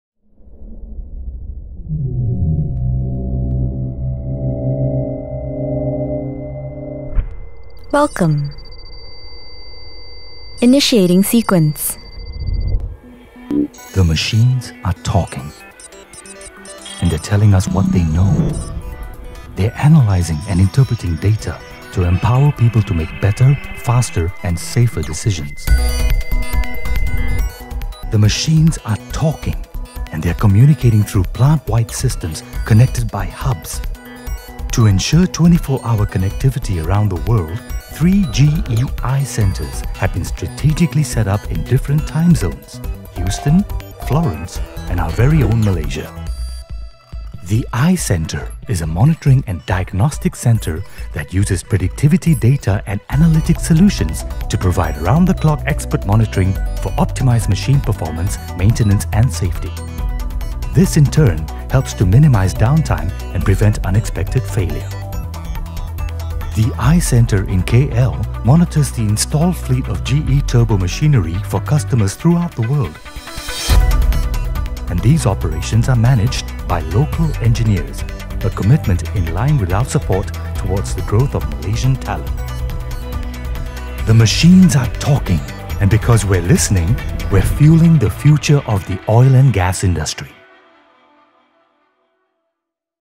Industrial Internet for Oil & Gas
The work and talent that goes behind the Remote Monitoring & Diagnostics Center or also know as iCenter, around the world and in Malaysia.

Kuala Lumpur is home to one of the three iCenter's around the world.